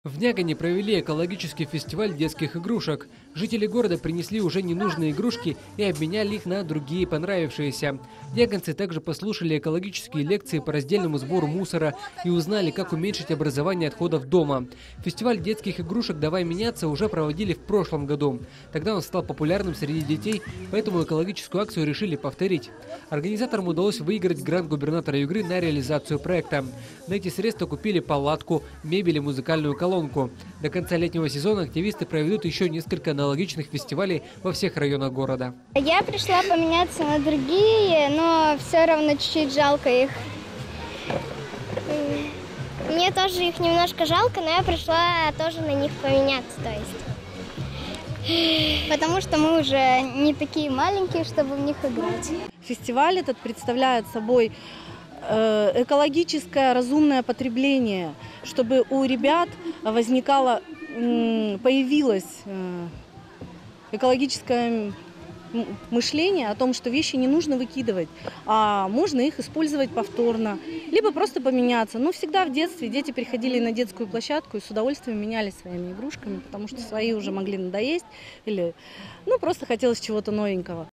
В Нягани провели экофестиваль игрушек
В Нягани провели экологический фестиваль детских игрушек. Жители города принесли уже ненужные игрушки и обменяли их на другие понравившиеся.